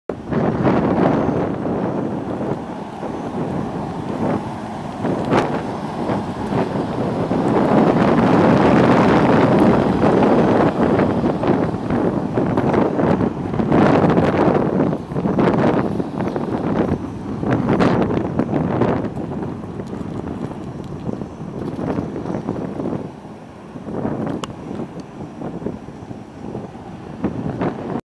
2000 Chevrolet Express 3500 van for sale at auction | bidding closes May 7, 2019
2000 Chevrolet Express 3500 van
133,792 miles on odometer
5.7L V8 OHV 16V gas engine
Automatic transmission
AC and heat
Tilt
Cruise
AM/FM
Power windows, locks, and mirrors
245/75R16 tires
Sale# 1924-KP
Unit# 861
Oklahoma title
1GCHG39R1Y1191306